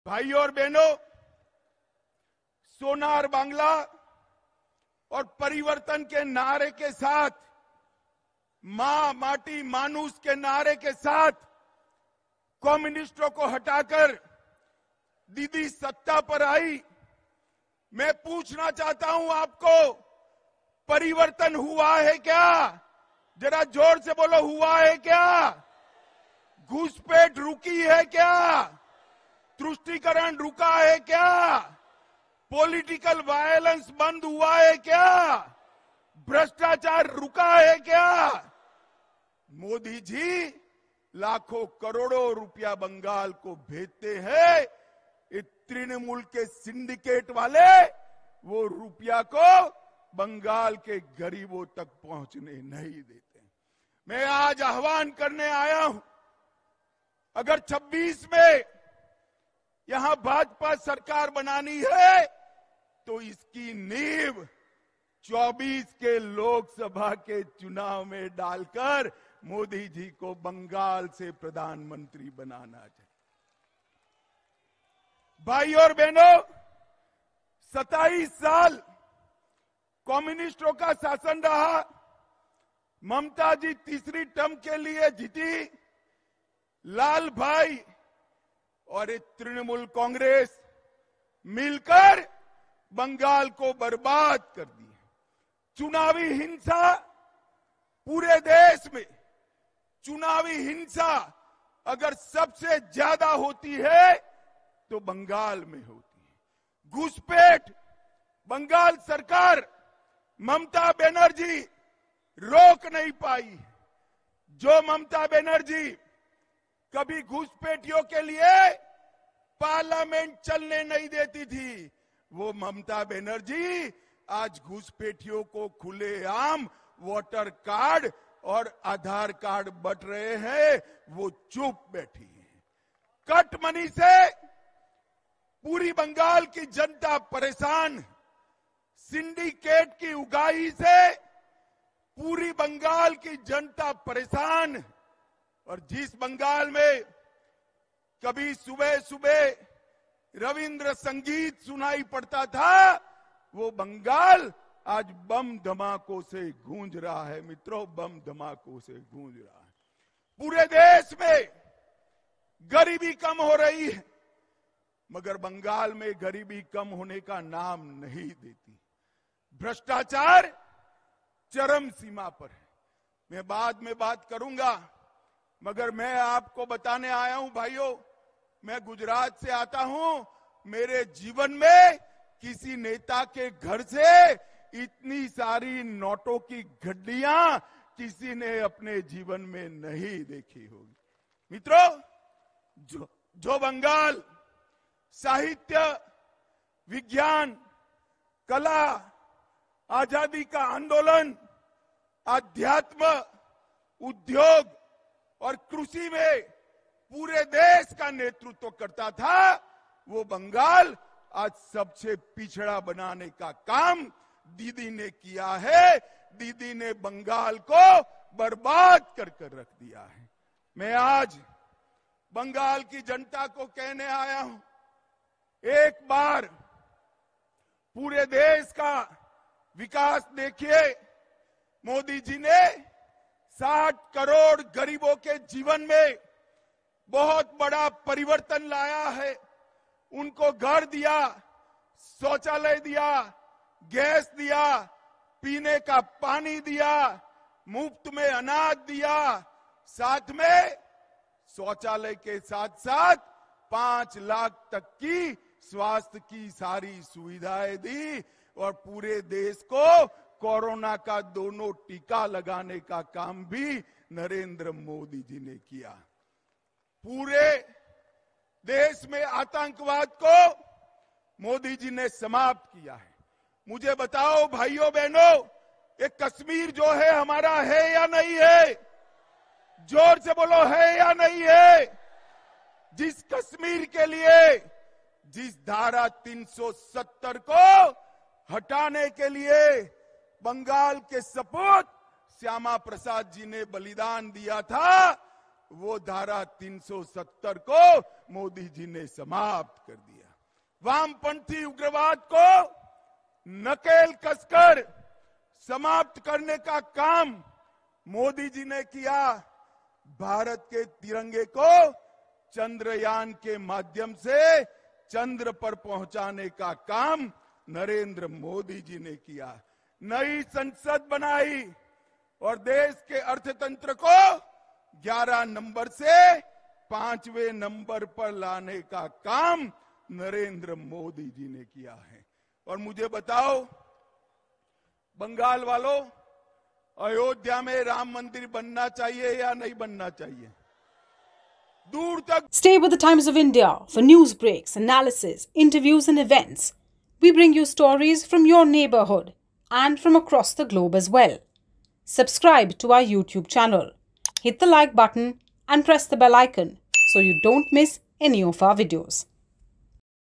Amit Shah: ‘BJP will win 2026 Bengal elections, CAA will be implemented’ | CM Mamata Banerjee
Union Home Minister Amit Shah accused Mamata Banerjee of ruining West Bengal, alleging the Trinamool Congress government's involvement in political violence, corruption, and appeasement politics. Speaking at a Kolkata rally, Shah asserted the BJP's intention to secure a two-thirds majority in the state after the 2026 assembly elections. He highlighted the reported killing of 212 BJP workers in the state, asserting that the people would retaliate through votes in the upcoming elections, portraying the political landscape as a battleground for change and retribution. “Infiltration in the state is at its peak… People post on social media to make Indian documents available to intruders… Let me tell you, CAA is a law of the nation and nobody can stop it… We will implement CAA in Bengal,” said Amit Shah.